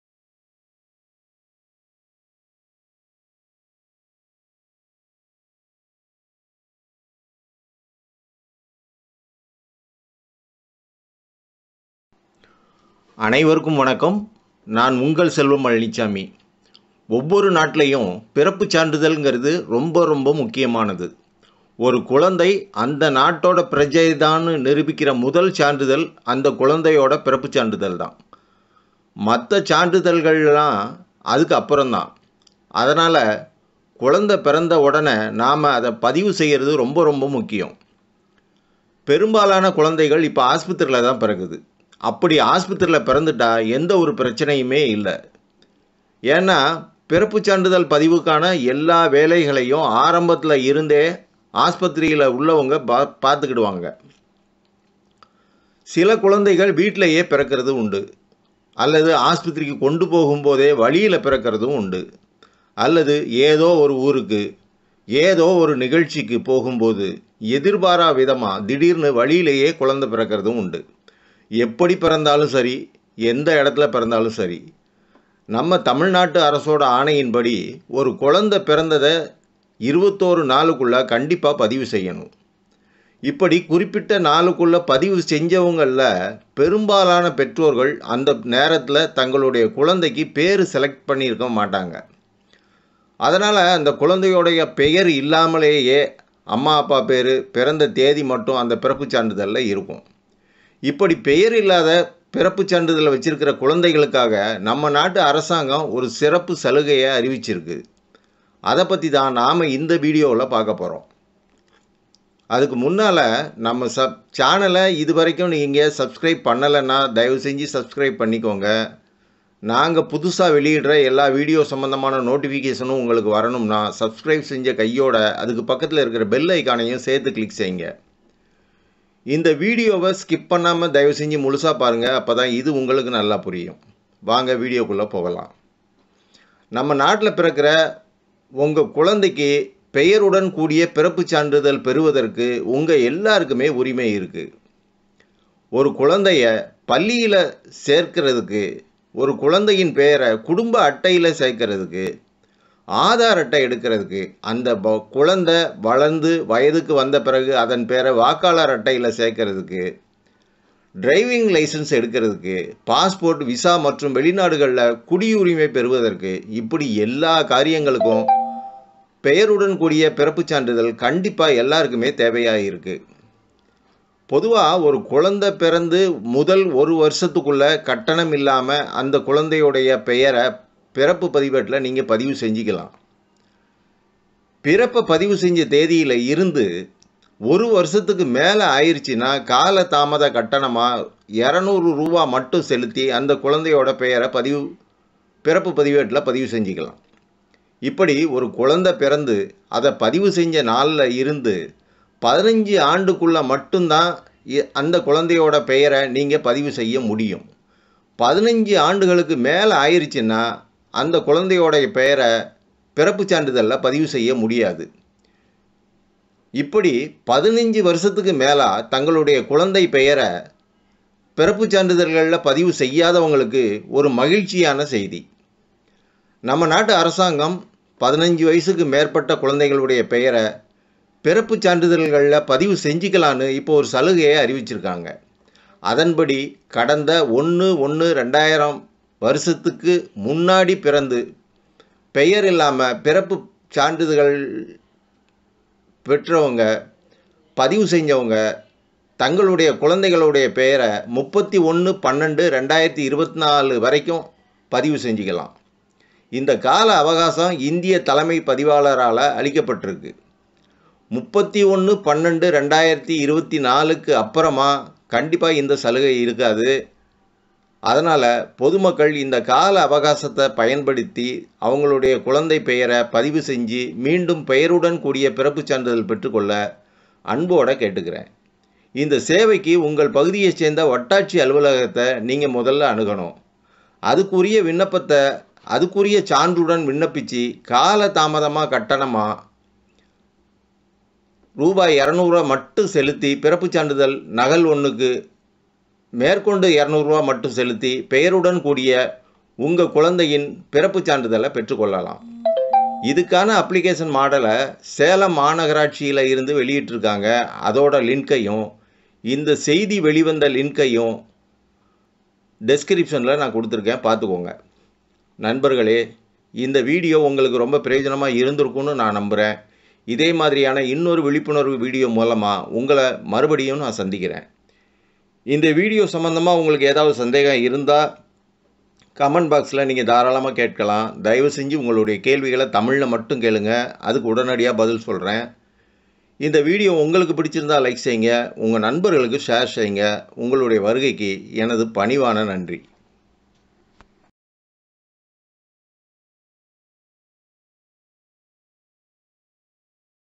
225. உங்கள் பெயரை, பிறப்புச் சான்றிதழில் இன்னும் பதிவு செய்யவில்லையா? வந்திருக்கு ஒரு வாய்ப்பு!
உங்கள் பெயரை, பிறப்புச் சான்றிதழில் இன்னும் பதிவு செய்யவில்லையா? வந்திருக்கு ஒரு வாய்ப்பு!